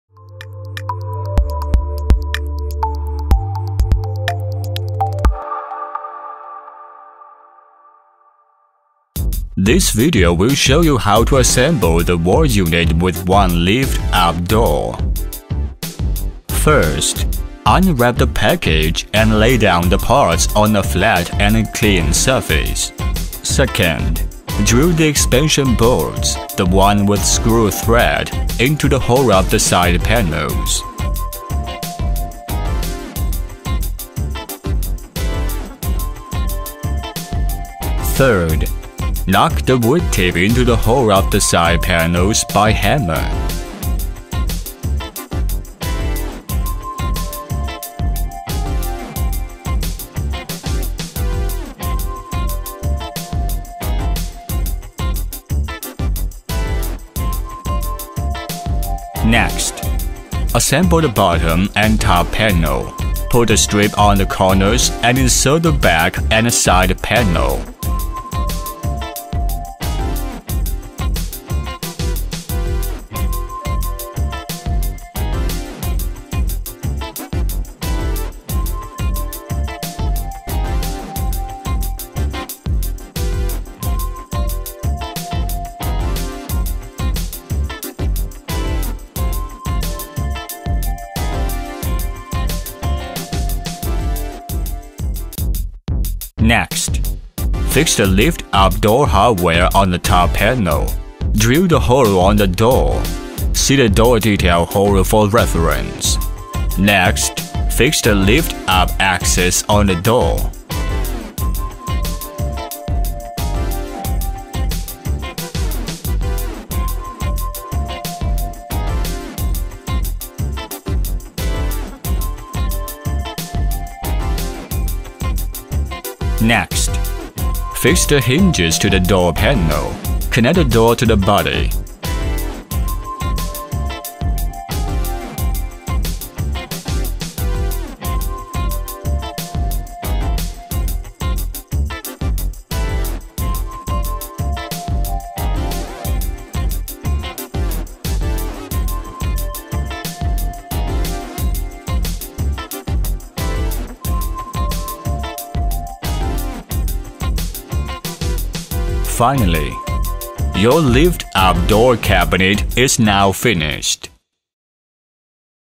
Lift-Up Door Wall Cabinet Assembly Guide (WL3015-- TLS-001A1)
This video will show you how to assemble the Wall Unit with 1 Lift Up door.
The box should contain: bottom panel, side panel, strips, back panel, accessories and lift-up hardware.

1. Unwrap the package and lay down the parts on a flat and clean surface.

2. Drill the expansion bolts (the one with screw thread) into the hole of the side panels.

3. Knock the wood tip into the hole of the side panels by hammer.

4. Assemble the bottom and top panel. Put the strip on the corners and insert the back and side panel.

5. Fix the lift-up door hardware on the top panel. Drill the hole on the door (see the door detail hole for reference).  Next, fix the lift-up access on the door.

6. Fix the hinges to the door panel. Connect the door to the body.

7. Your Lift-up door cabinet is now finished!